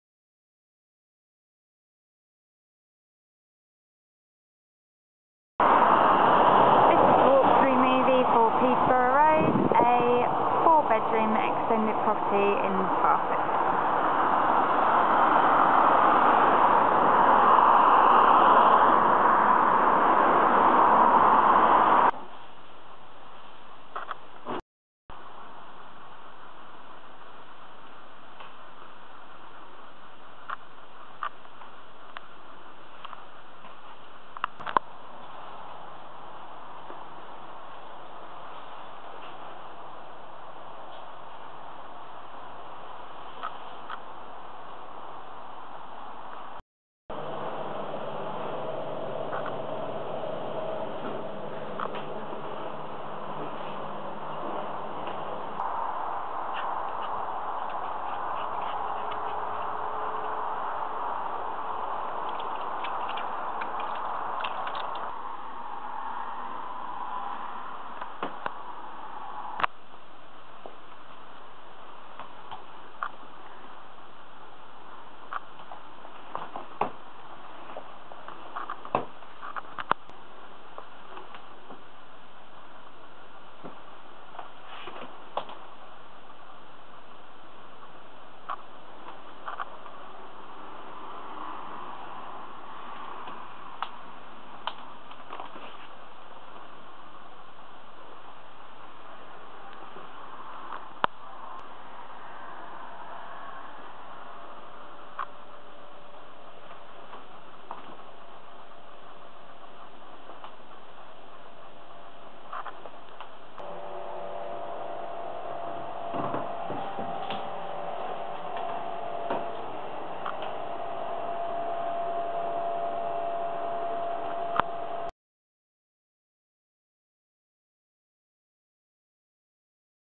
Immaculate unfurnished 4 bed family home with large living space
Immaculate unfurnished 4 bed family home with large living space, 2 reception rooms and downstairs shower room. 2 double bedrooms and 2 singles. Family bathroom upstairs. Large enclosed garden, garage. GCH